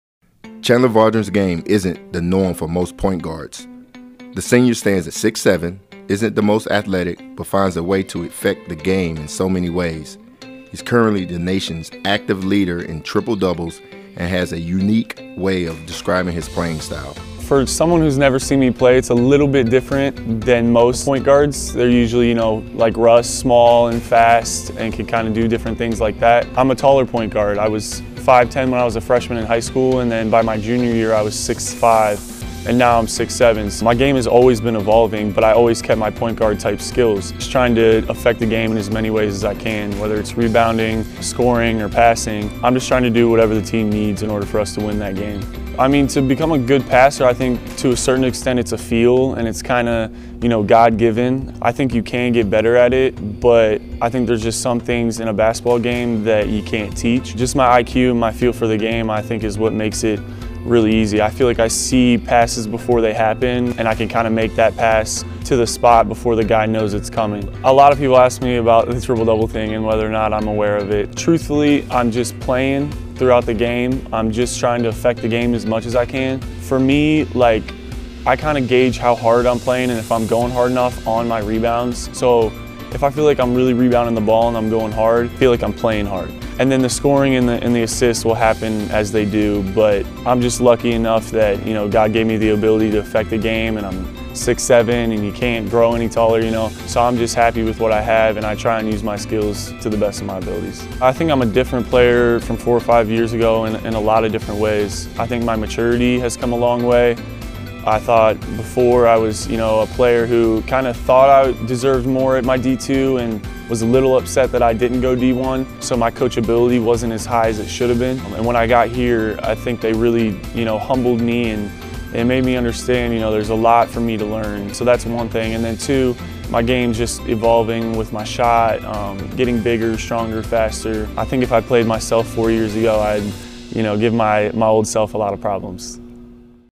Men's Basketball | Chandler Vaudrin Exclusive (Part 2)
Chandler Vaudrin chats about his style of play.